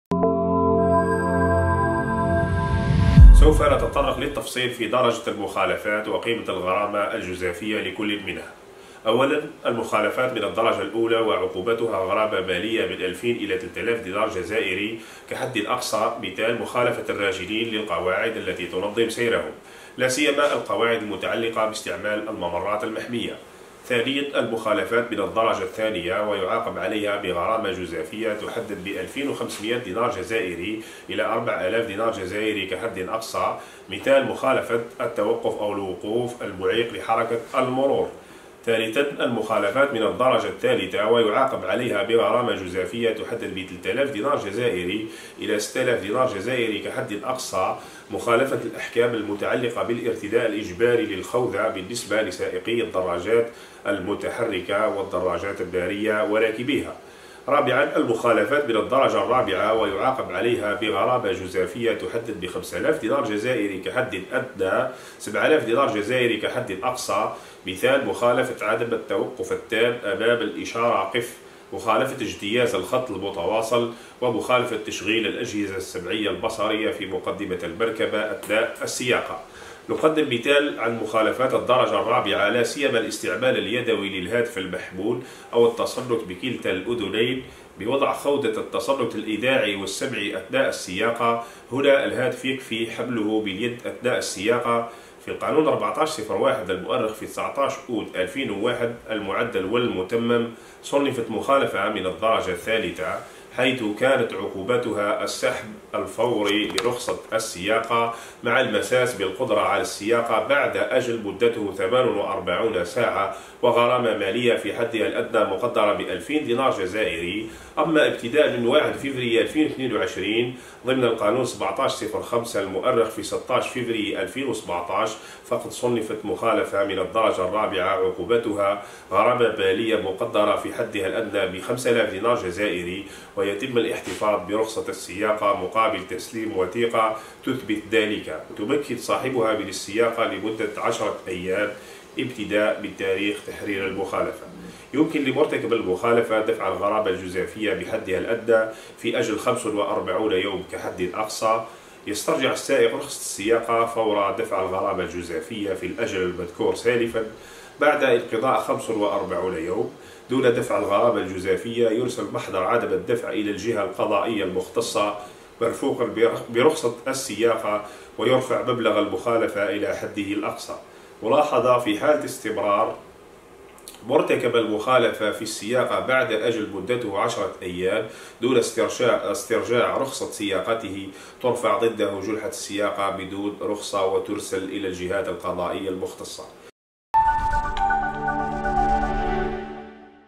توضيح مخالفة بعض الأحكام الواردة في قانون المرور 01-14 المعدل والمتمم
الجزائر (وأج) -  توضيح مخالفة بعض الأحكام الواردة في قانون المرور 01-14 المعدل والمتمم من قبل الرائد سمير بوشحيط من مركز الإعلام وتنسيق المرور للدرك الوطني.